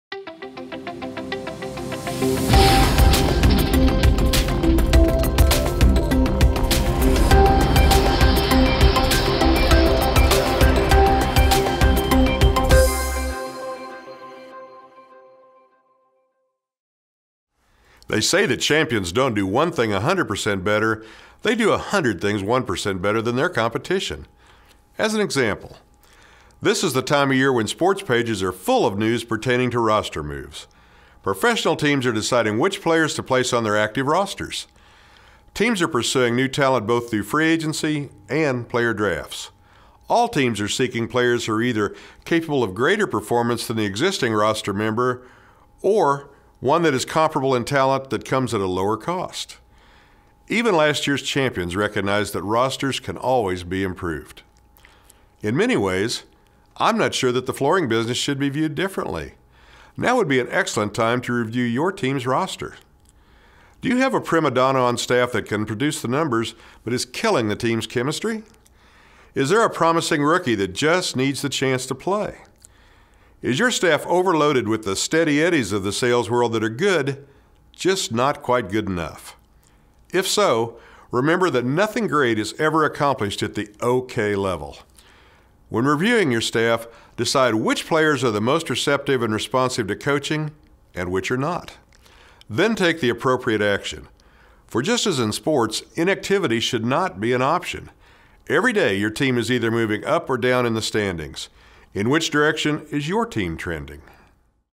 REVIEW YOUR ROSTER
Tom Jennings, the VP of Professional Development for the WFCA, presents short video tips for improving your customer service and optimizing your staff performance. In the end, it’s all about understanding the importance of being 1% better than your competition.